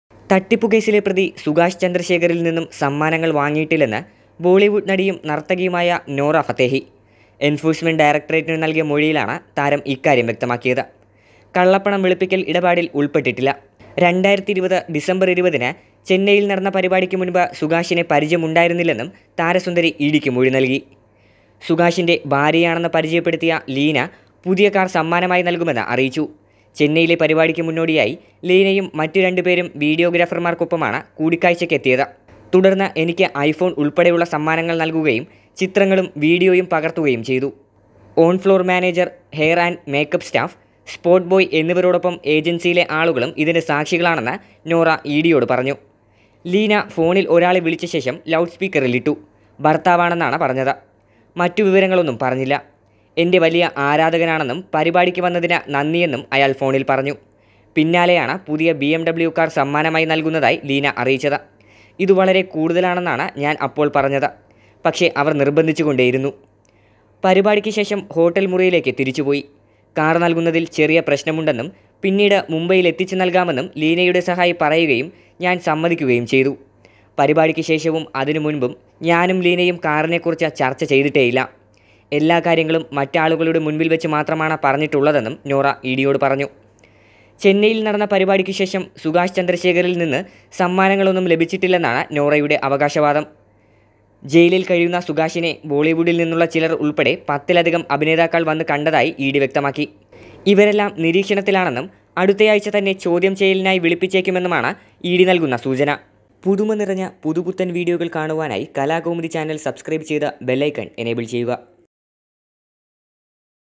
തട്ടിപ്പുകേസിൽ നോറ ഫത്തേഹിയുടെ മൊഴി പുറത്ത് Nora Fatehi's Link Surfaces|Bling, Blackmail & Bollywood
Nora Fatehi received gifts worth crores from conman Sukesh Chandrashekhar. Is there more to it than meets the eye? Conman Sukesh Chandrashekhar spent crores of rupees on Bollywood actress Nora Fatehi with money he extorted and gifted her a BMW car.